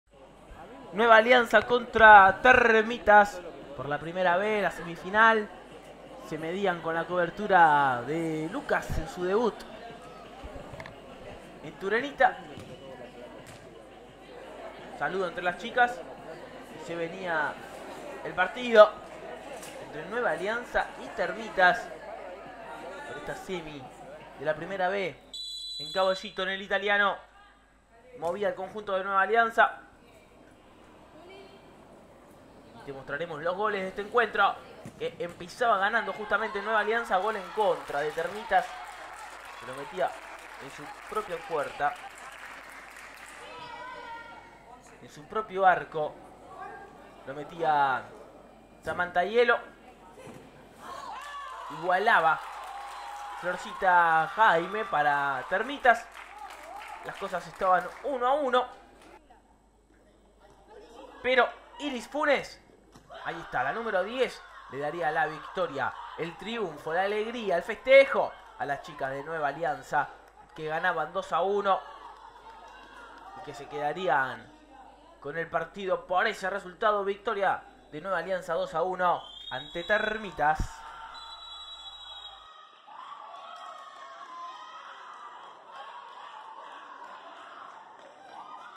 Semifinal Torneo de Verano: Nueva Alianza - Termitas
Estuvo peleadísima la semi entre Nueva Alianza y Termitas. Desde el comienzo le costó a ambos equipos imponerse, pese a tener chances para convertir. Fueron las de celestes quienes consiguieron ponerse en ventaja, y las de blanco lo igualaron. El encuentro parecía que se iba a penales, hasta que las de Nueva Alianza volvieron a ponerse al mando y se aseguraron un lugar en la final.